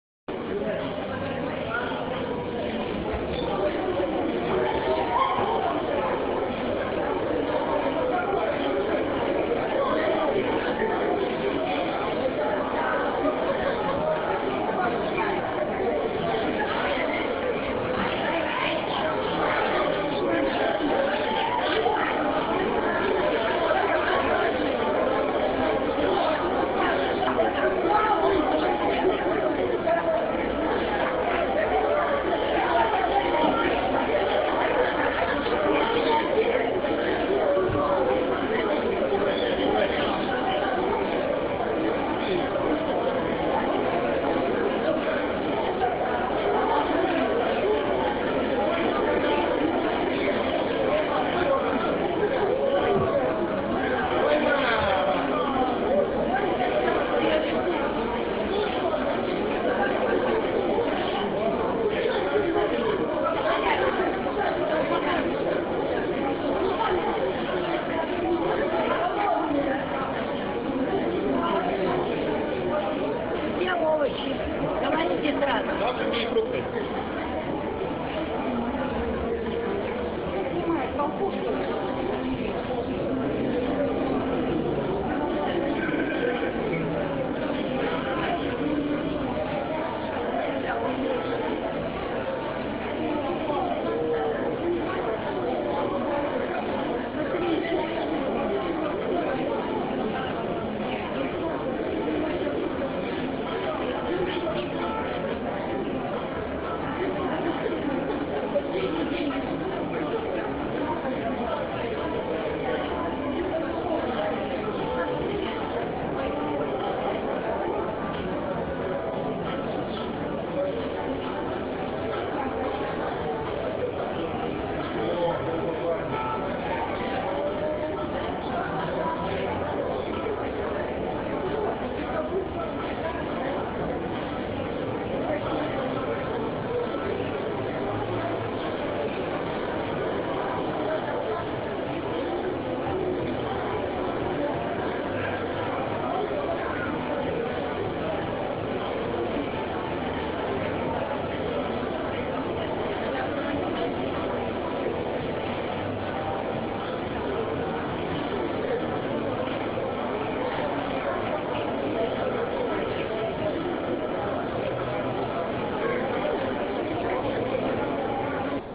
altapress.ru: Открытие ТЦ "Пионер" в Барнауле
На открытие "Пионера" и "Ленты" пришли толпы барнаульцев.